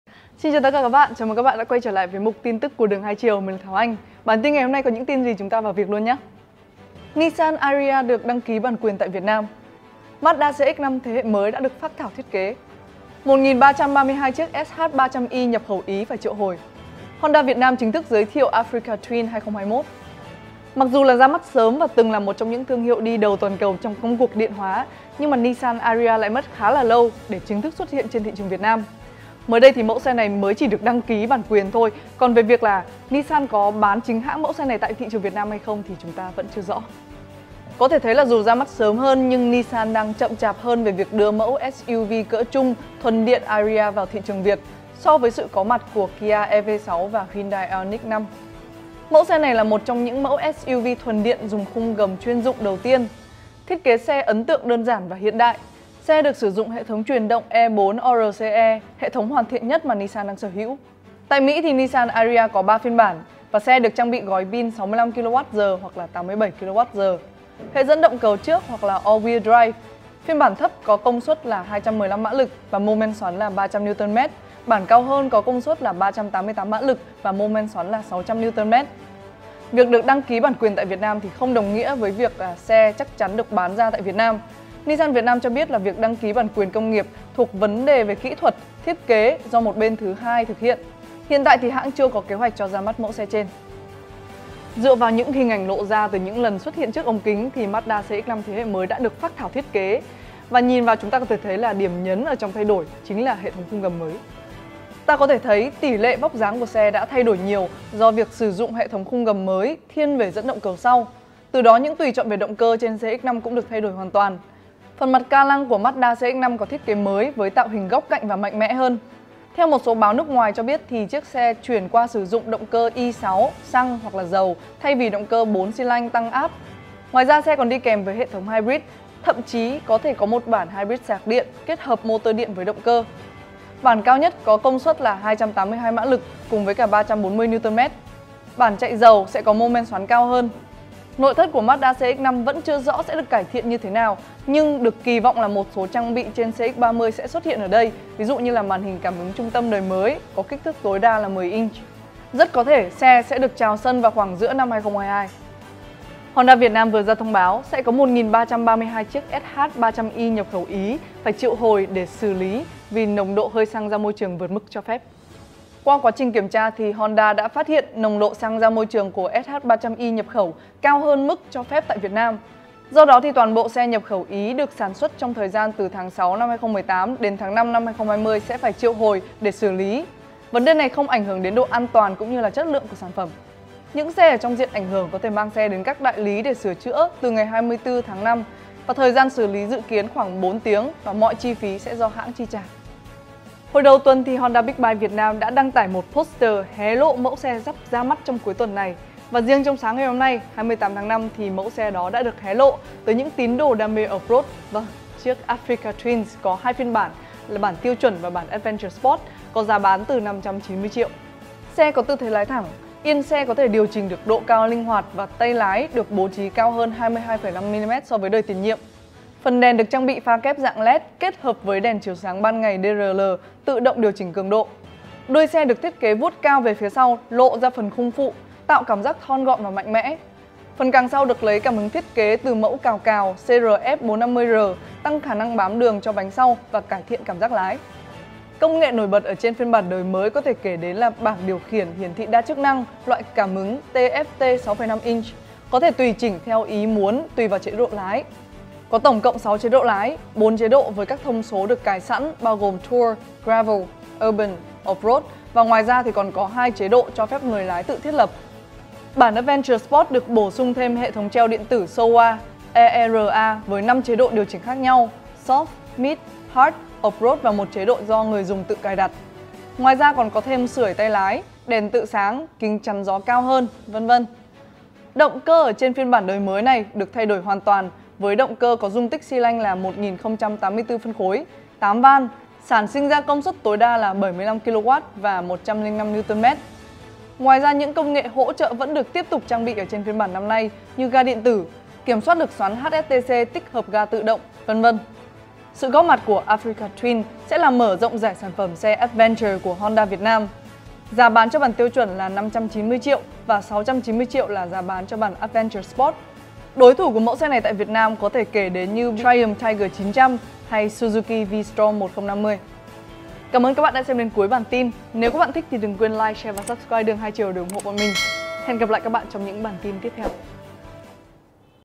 Mazda Cx-5 mới có bản phác thảo, Honda Africa Twin 2021 ra mắt, triệu hồi Sh300i | Đường 2 chiều.
Chúc cả nhà cuối tuần vui vẻ! Cùng điểm qua những điểm nhấn về tin tức xe cộ trong tuần này cùng Thảo Anh nhé:
Nissan Ariya được đăng ký bản quyền tại Việt Nam.
Mazda Cx-5 thế hệ mới đã được phác thảo thiết kế.
1332 chiếc SH300i nhập khẩu Ý phải triệu hồi.
Honda Việt Nam đã chính thức giới thiệu Africa Twin 2021 với 2 phiên bản: tiêu chuẩn và Adventure Sport .

☎️ Mr. Huy - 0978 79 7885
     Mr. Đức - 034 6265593